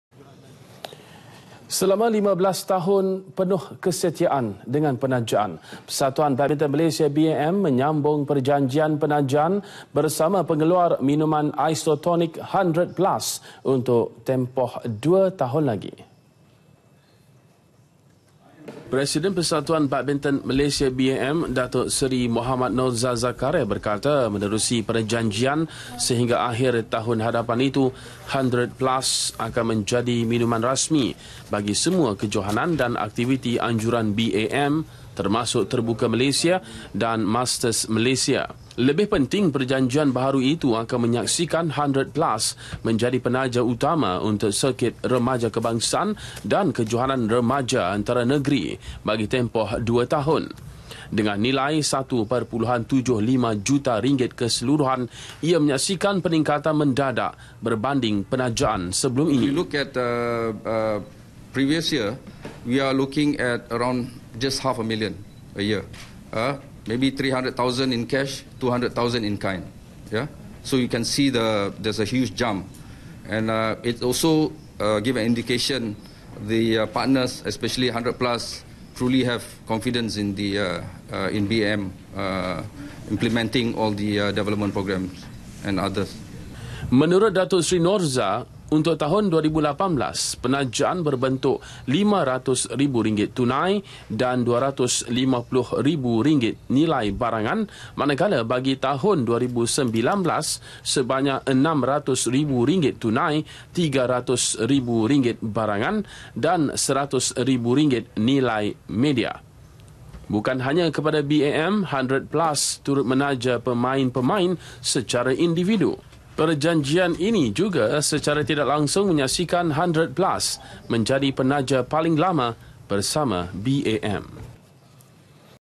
BAM SAMBUNG PERJANJIAN PENAJAAN BERSAMA 100 PLUS UNTUK TEMPOH LAGI 2 TAHUN-8PM [06 MAC 2018]
Selama 15 tahun penuh kesetiaan dengan penajaan,  Persatuan Badminton Malaysia BAM menyambung perjanjian penajaan bersama pengeluar minuman isotonik, 100 plus untuk tempoh lagi 2 tahun.
Presiden Persatuan Badminton Malaysia BAM, Datuk Seri Mohamad Norza Zakaria berkata menerusi perjanjian sehingga akhir tahun hadapan itu, 100 plus akan menjadi minuman rasmi bagi semua kejohanan dan aktiviti anjuran BAM termasuk Terbuka Malaysia dan Masters Malaysia.